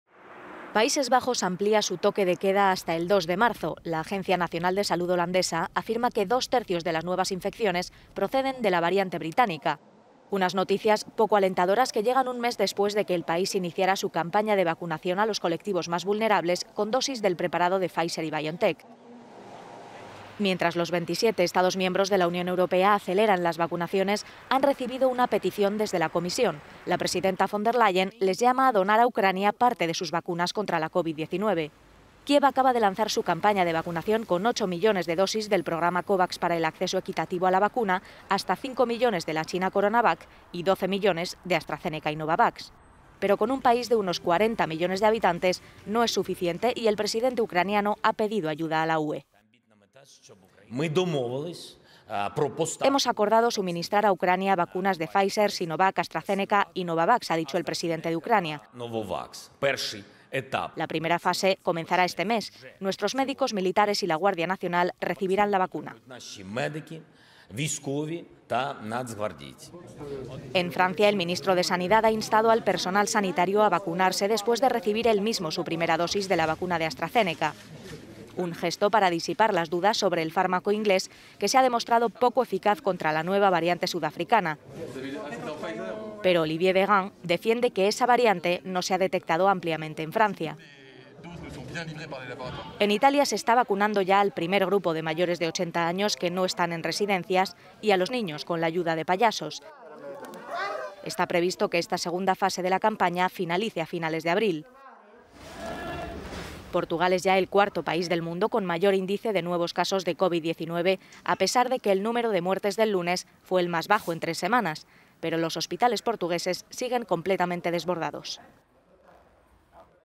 Ucrania lanza su campaña de vacunación pidiendo viales a sus vecinos europeos
Mientras Europa acelera sus campañas de vacunación, la Comisión Europea pide a los Estados miembros que donen parte de sus viales a Ucrania